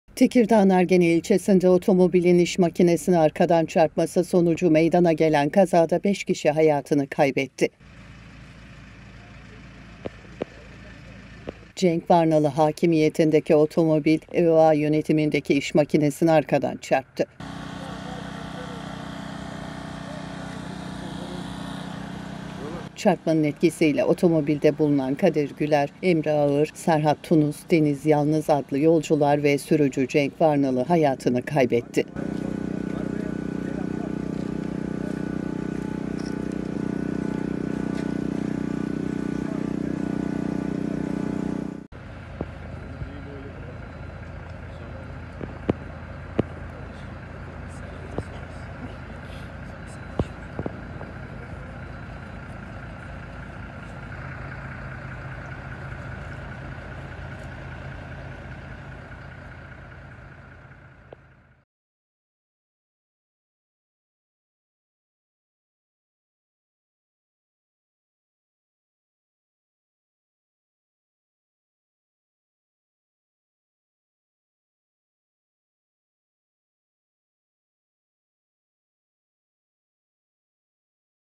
Tekirdağ'da Trafik Kazası, Otomobil İş Makinesine Arkadan Çarptı : 5 Ölü
Tekirdağ’ın Ergene ilçesinde otomobilin iş makinesine arkadan çarpması sonucu meydana gelen kazada 5 kişi hayatını kaybetti.

Kaza, Tekirdağ’ın Ergene ilçesi Edirne yolu Ulaş Mahallesi girişinde gece saatlerinde meydana geldi. Cenk Varnalı hakimiyetindeki 59 FV 888 plakalı otomobil, Ö.A. yönetimindeki iş makinesine arkadan çarptı. Çarpmanın etkisiyle otomobilde bulunan Kadir Güler, Emre Ağır, Serhat Tunus, Deniz Yalnız adlı yolcular ve sürücü Cenk Varnalı hayatını kaybetti.